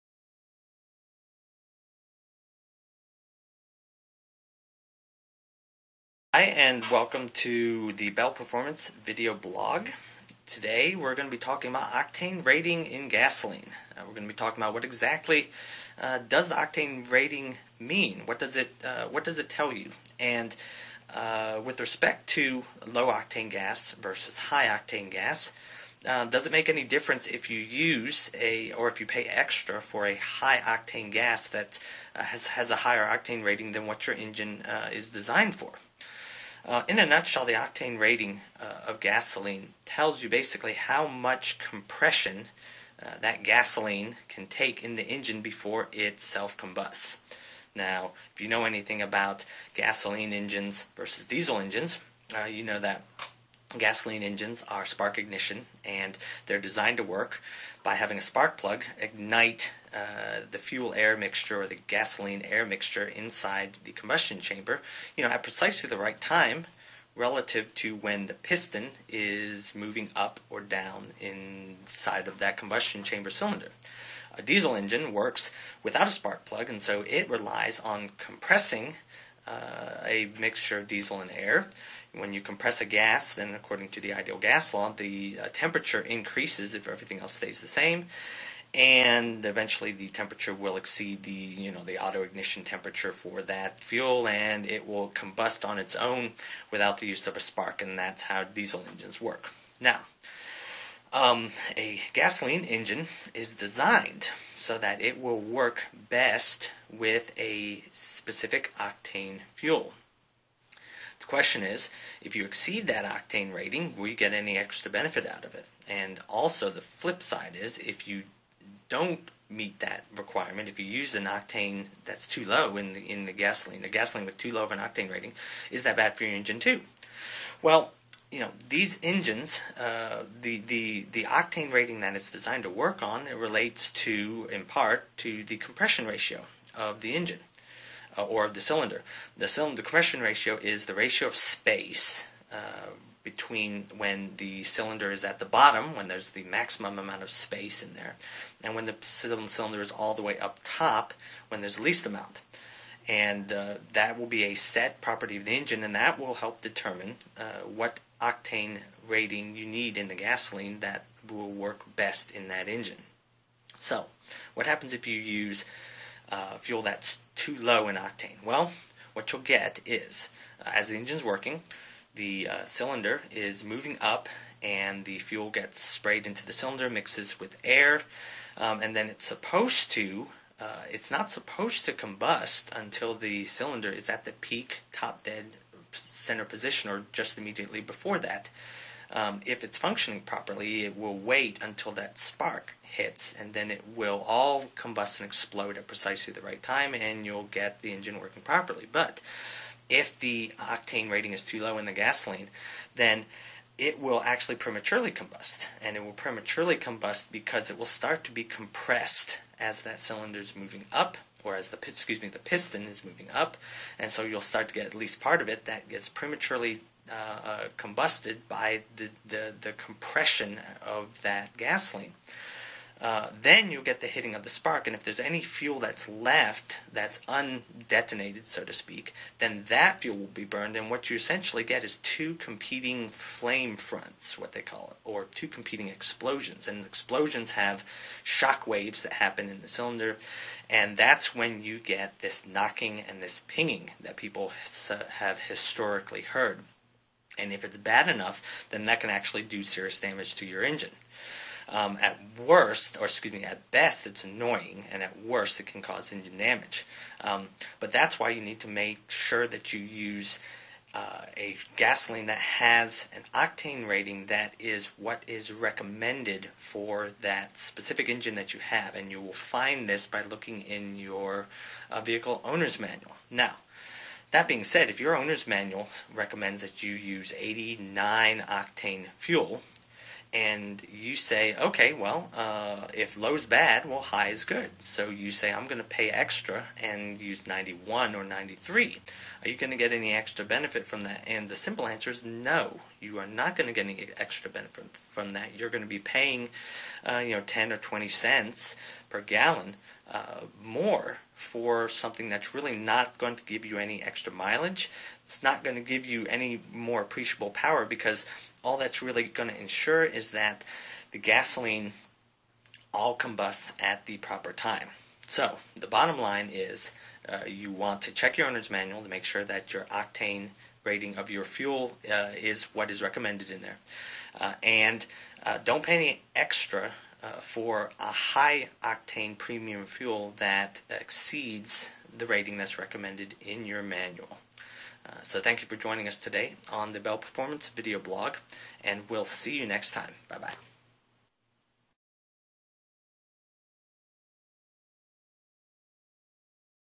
What is Octane Rating in Gas | Bell Performance | Video Blog
Do you have to buy high octane gas to get better mileage? What does high octane rating in gas do? The Bell Performance Video Blog discusses.